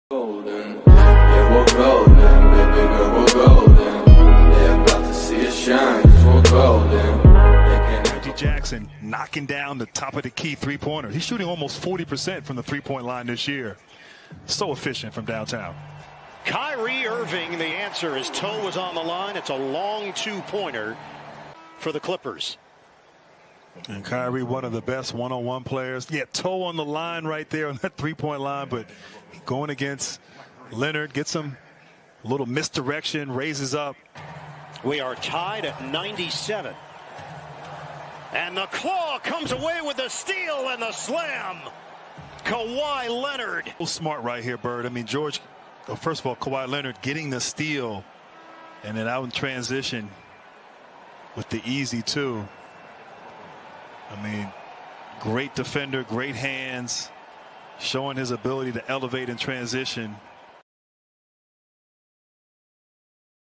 Kyrie Crosses up Leonard but Kawhi got a revenge on 4th Quarter
We might choose it and give you a Shoutout!!